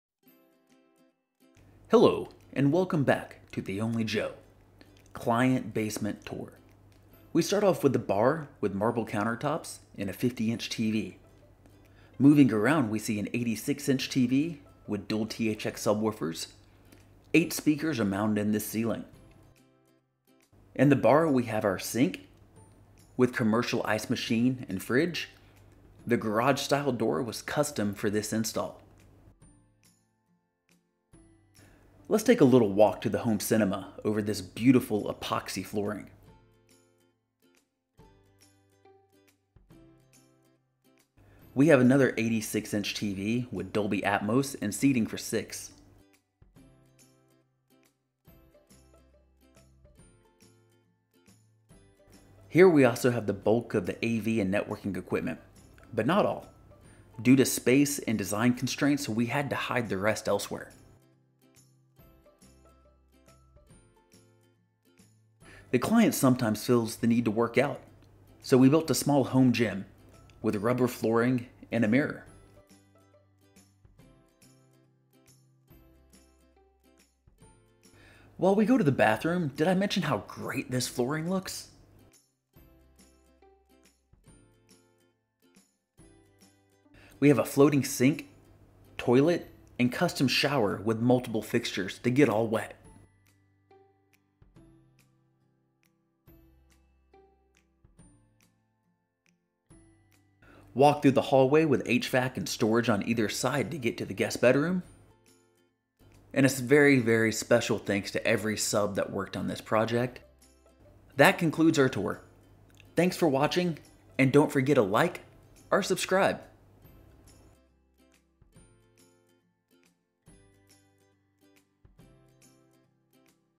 The Ultimate Basement Walk-through
We cap off nearly three months of construction mayhem to deliver the (almost) final product to our client. We began this project three months ago with a blank canvas and a client that was open to my crazy ideas.

A special thanks goes out to all of the sub-contractors and skilled workers who stuck it out through lock-downs, my yelling, and constant design changes to finish in time for the 4th of July.